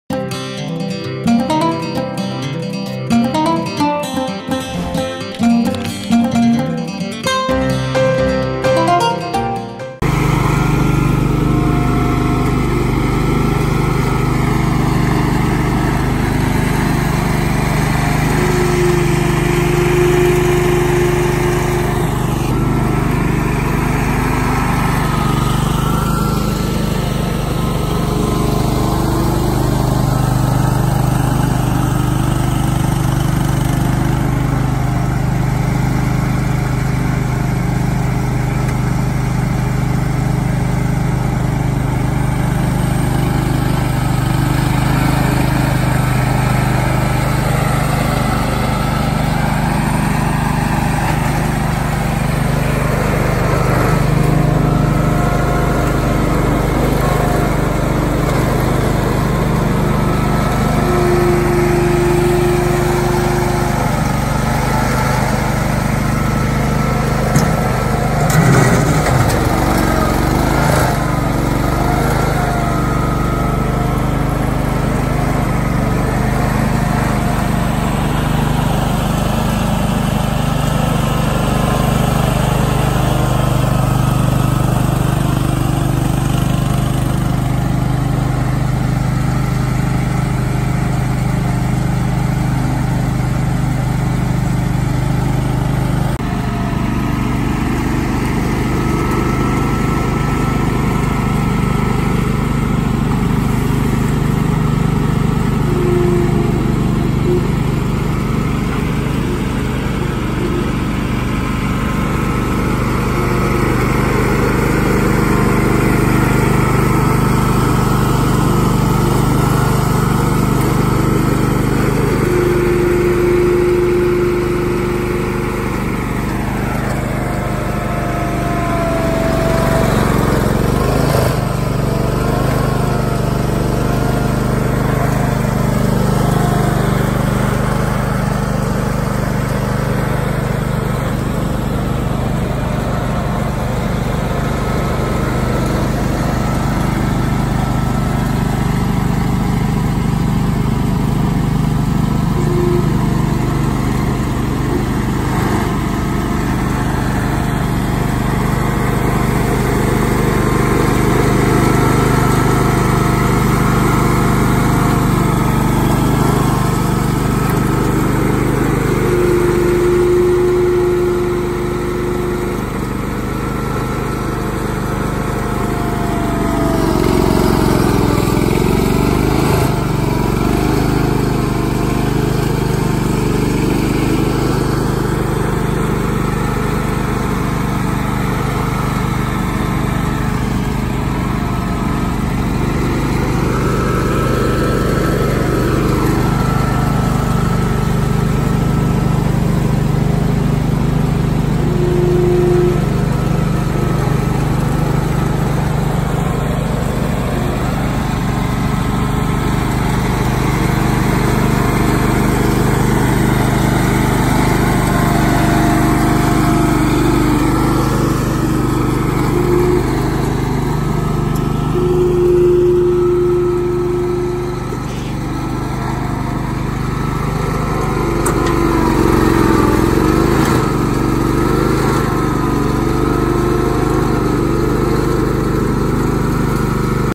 Bolens 1250 w/ Loader
Another video from 8/2019, of our Bolens 1250 being put to work moving wood chips.